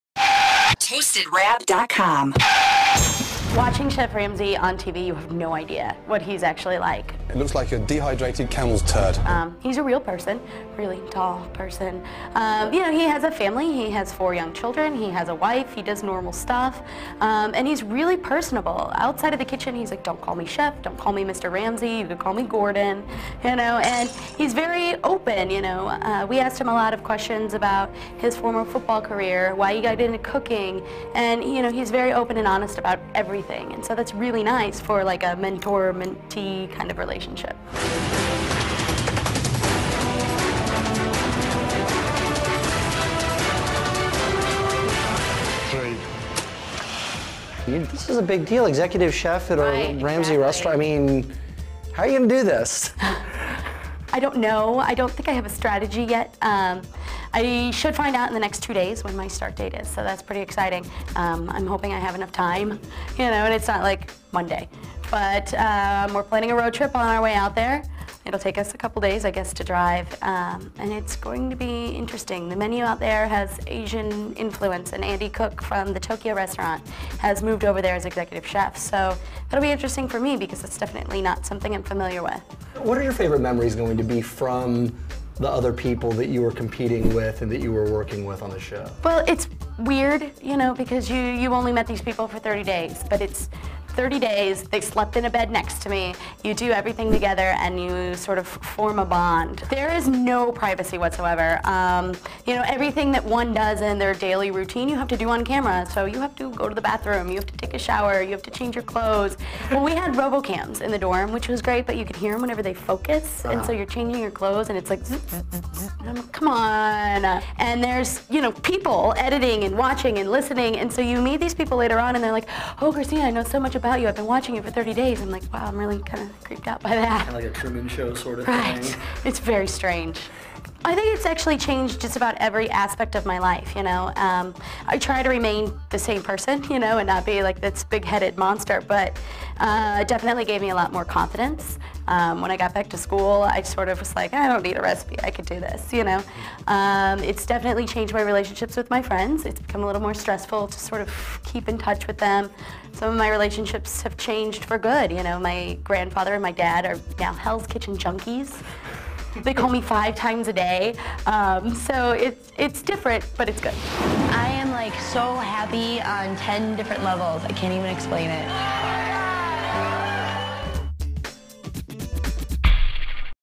Hells Kitchen Winner Christina INTV
an interview with christina, st. louisan, and winner of hells kitchen 4